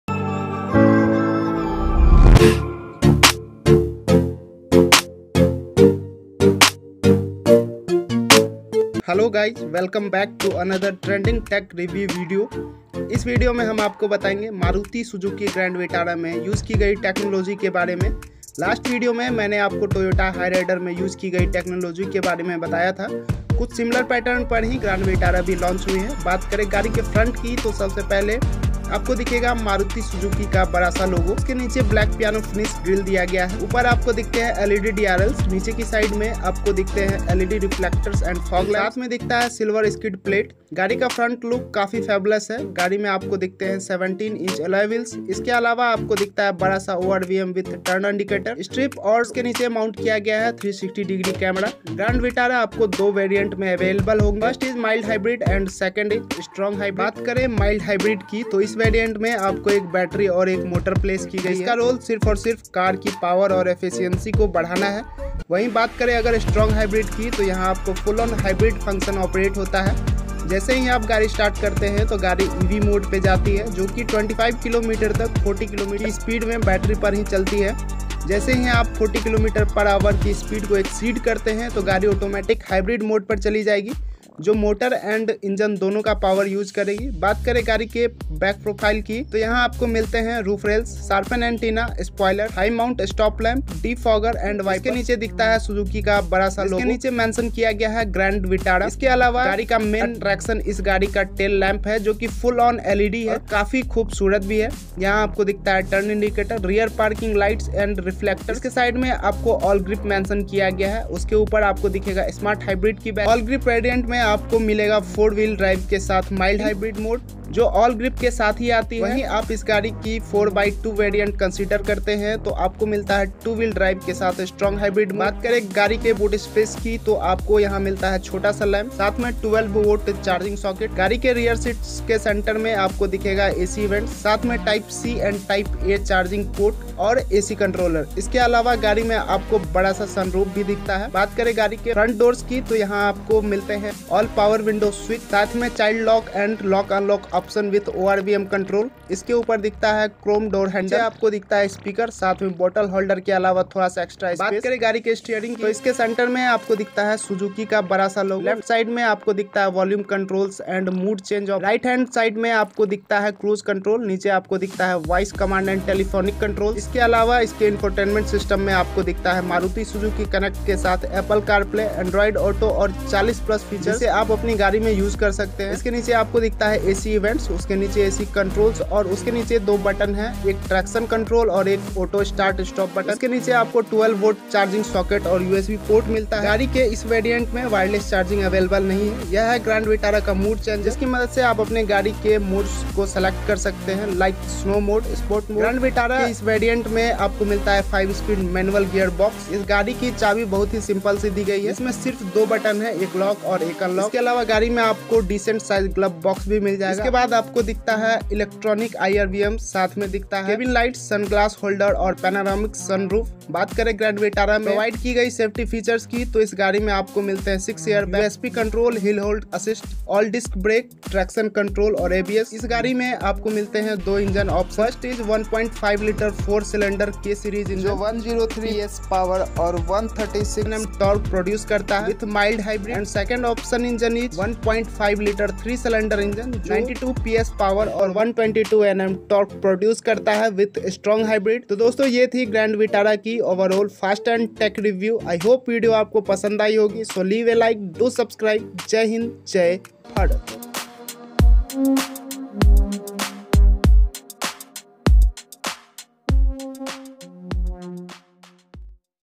||GRAND VITARA 2022 FAST AND TECH REVIEW BY TRENDING TECHNOLOGY||
Video Credit _Rajni Chaudhary (DiDi)
In this video Maruti Suzuki Grand vitara Fast and Tech review by Trending Technology . this video Created by Nischal Chauhan For Knowledge purpose only.
Maruti Suzuki Grand vitara.
I am not the owner of the pictures and images Thanks.